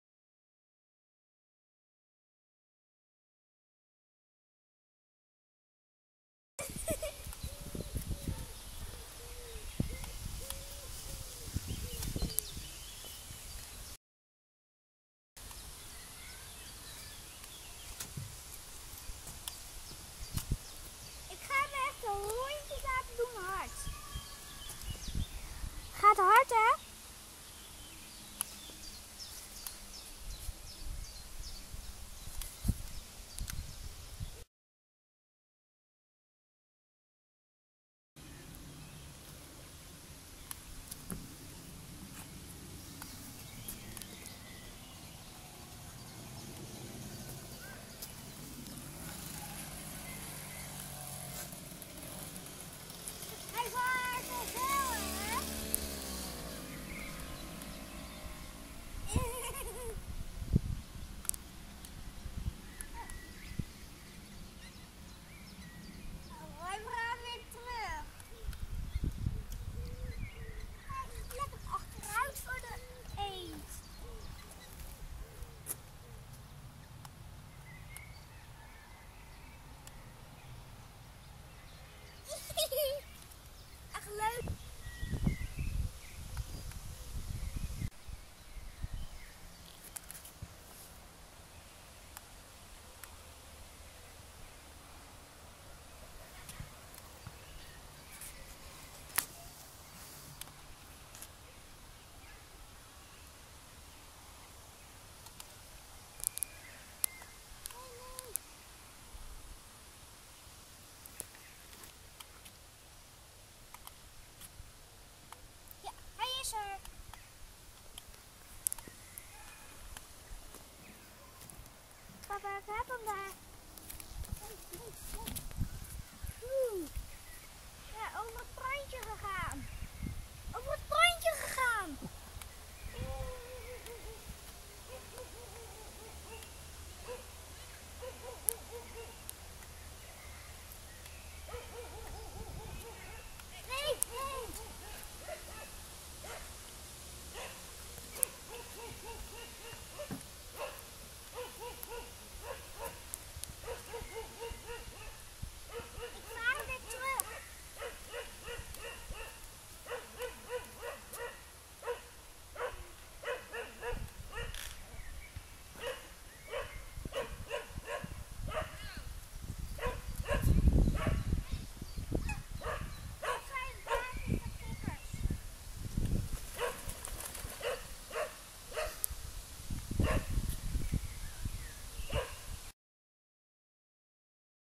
Thijs en zijn RC EEND
Tuinvijvereend van Intratuin omgebouwd met de electronica uit een Dickie speedbootje.